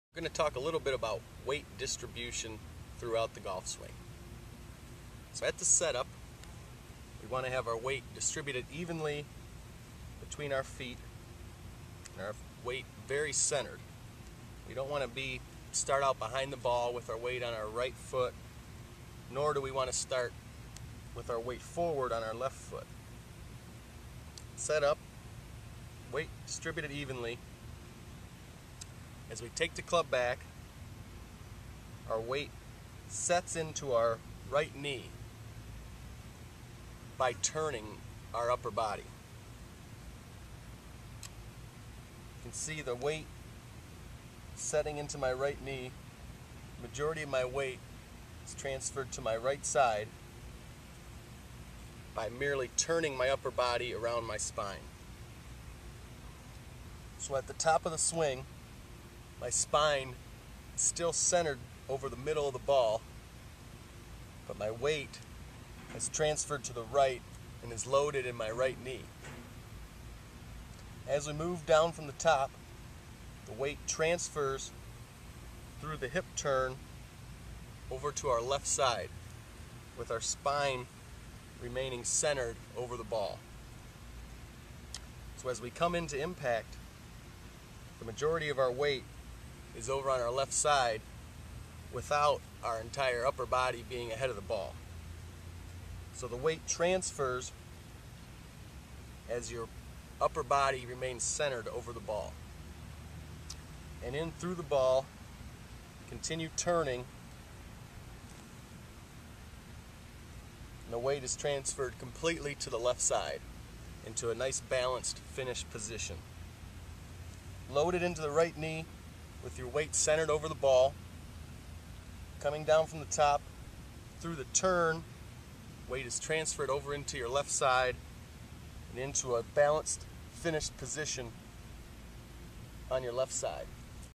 Golf Lesson Weight Transfer
A quick lesson on a proper weight transfer in the swing.